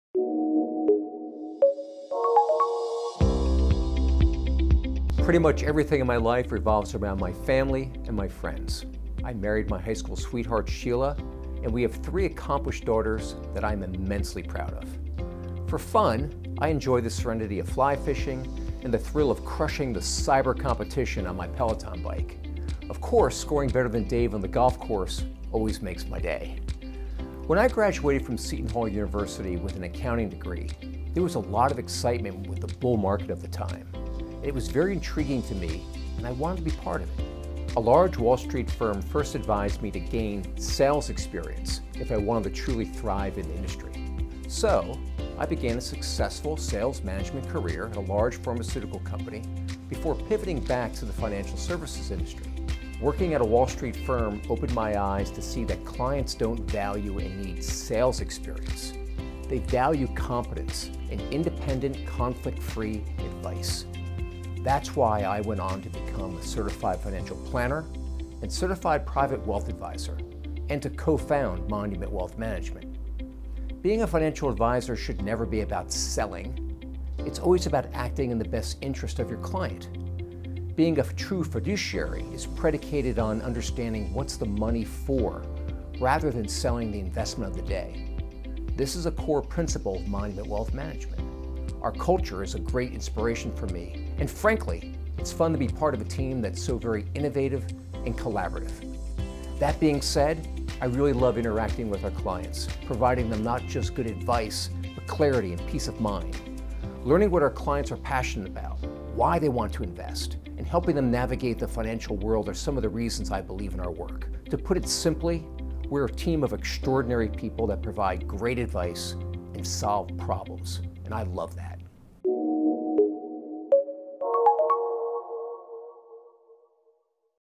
Dean J. Catino, CFP®, CPWA® | President & Co-Founder
Pretty much everything in my life revolves around my family and friends. I married my high school sweetheart, Sheila. and we have three accomplished daughters that I am immensely proud of. For fun, I enjoy the serenity of fly fishing and the thrill of crushing the “cyber competition” on my Peloton bike. …Of course, scoring better than Dave on the golf course always makes my day!

When I graduated from Seton Hall University with an accounting degree, - there was a lot of excitement with the bull market at that time --- it was very intriguing to me and I wanted to be a part of it.   A large Wall Street firm advised me to “first” gain “sales experience” if I wanted to truly thrive in the industry. So - I began a successful sales management career at a large pharmaceutical company before pivoting to the financial services industry.

Working at a Wall Street firm opened my eyes to see that clients don’t value and need sales experience, they value competence and independent, conflict free advice. That’s why I went on to become a CERTIFIED FINANCIAL PLANNER and Certified Private Wealth Advisor…. And to co-found Monument Wealth Management.

Being a Financial Advisor should never be about selling… it’s about always acting in the best interest of your client. Being a true fiduciary is predicated on understanding “What’s the Money For” rather than selling the investment of the day. This is a core principle of Monument Wealth Management.

Our culture is a great inspiration for me and frankly --- It’s fun to be part of a Team that is so very innovative and collaborative.  That being said, I really love interacting with our clients, providing them not just good advice, but clarity and peace of mind. Learning what our clients are passionate about, why they want to invest and helping them navigate the financial world are some reasons why I believe in our work.  To put it simply, we are a Team of extraordinary people that provide great advice and solve problems... and I love that. Please see important video disclosure information